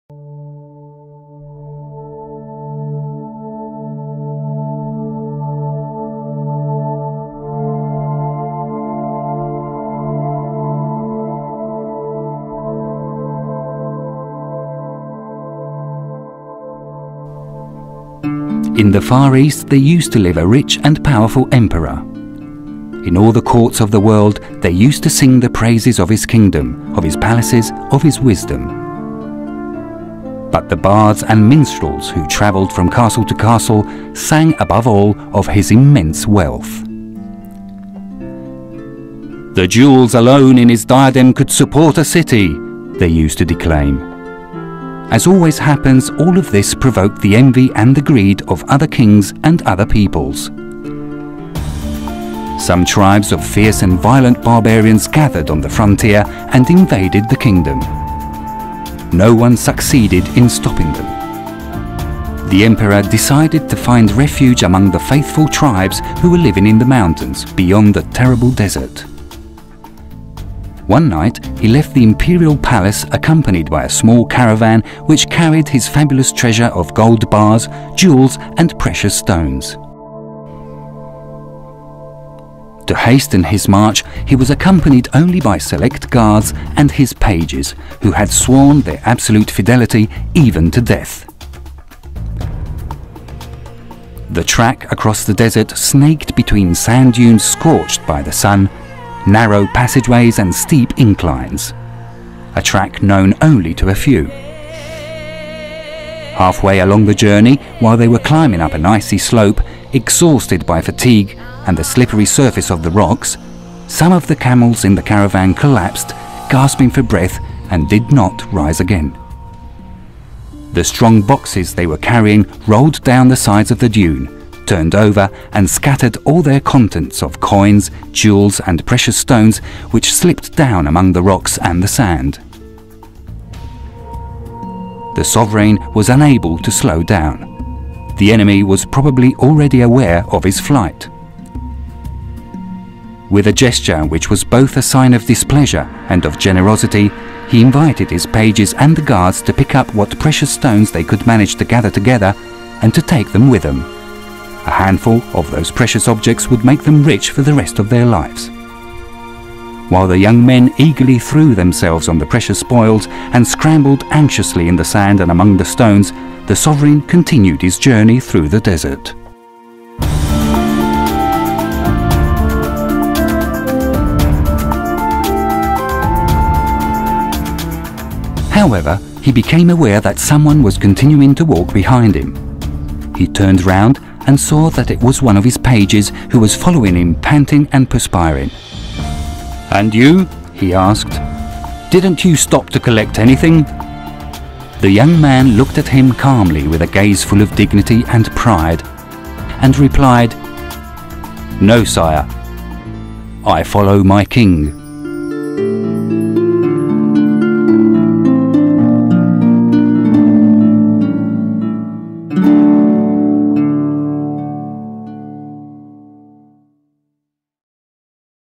Caravan in the Desert
A story excerpt from the 2011 Strenna of the Rector Major of the Salesians of Don Bosco